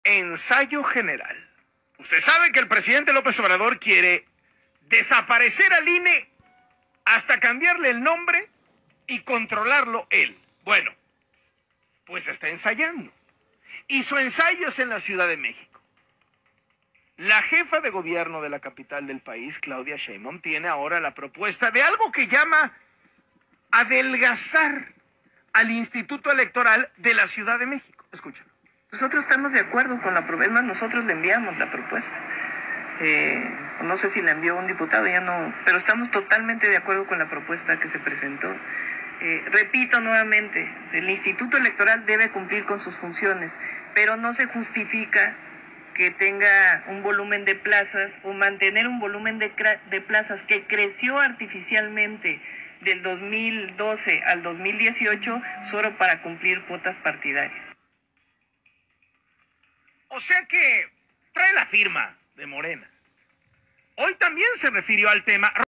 ALERTA NACIONAL! AMLO Y SHEINBAUM DESAPARECEN AL INE DE LA CDMX! LLEGÓ LA DICTADURA A LA CIUDAD!
Claudia SHEINBAUM ha desaparecido al Instituto Electoral de la Ciudad de México para ganar las elecciones del 2024 sin problema